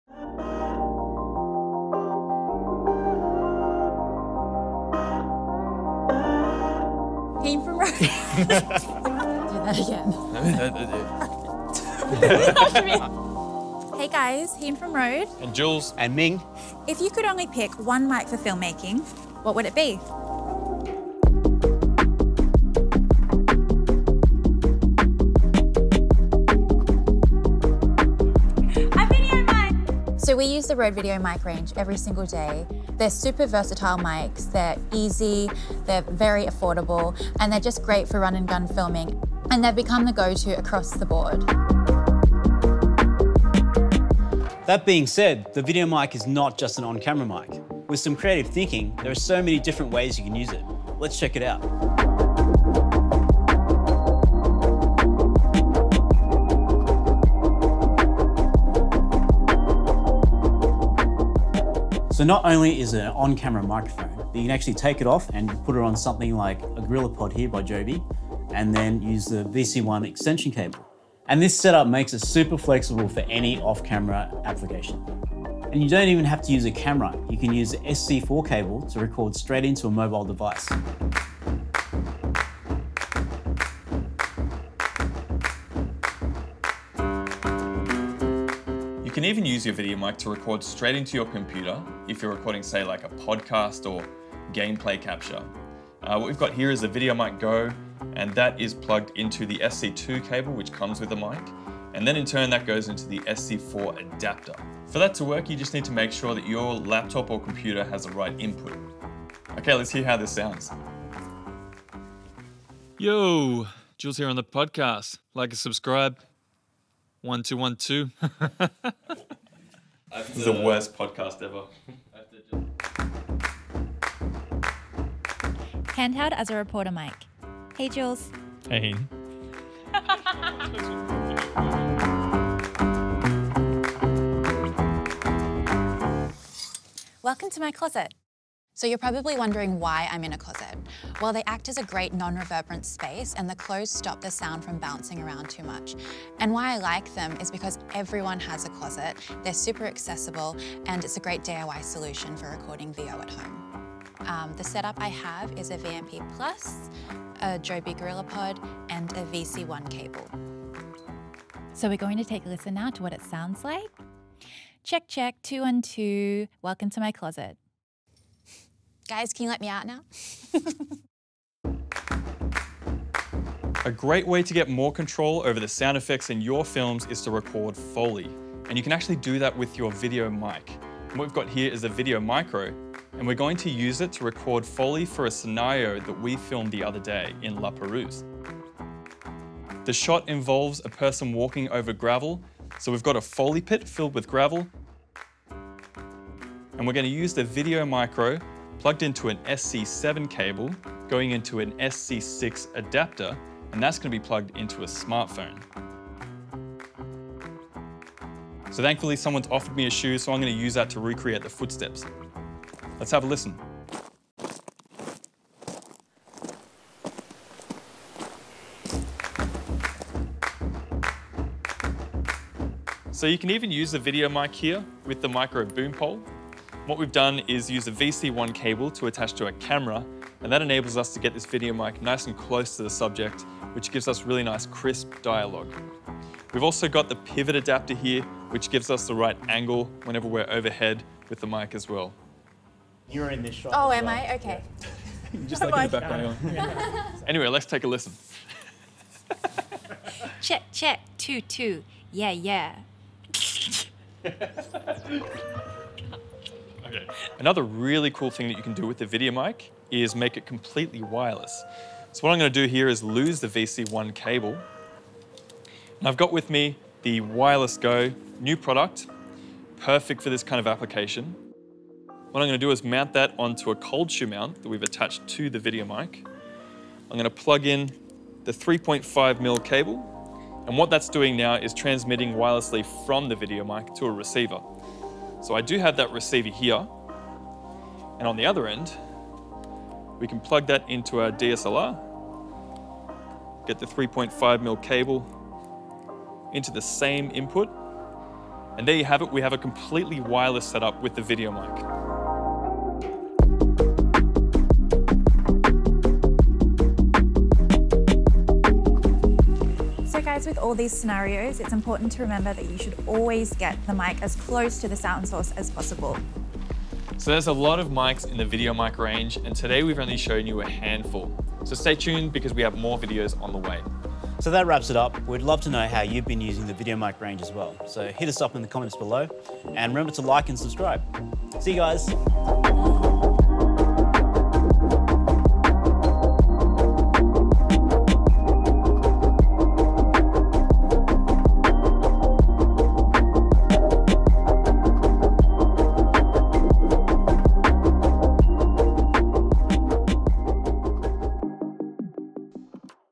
5 Other Uses For Your VideoMic | Audio for Film 101 - Episode 3
Welcome to Audio for Film 101! An all new series from RØDE Microphones, aimed at helping you get the best recording results for your next video or film.

This series will be helpful for any filmmaker that is looking to improve their audio or get the best out of their sound equipment, specifically microphones from the VideoMic range.

In this episode, Heen, Julian and Ming talk about how you could use your RØDE VideoMic in 5 unique recording situations!

For more in-depth info on this topic check out our full blog post

Join us on this series, as we share tips, tricks and hacks to get better sound!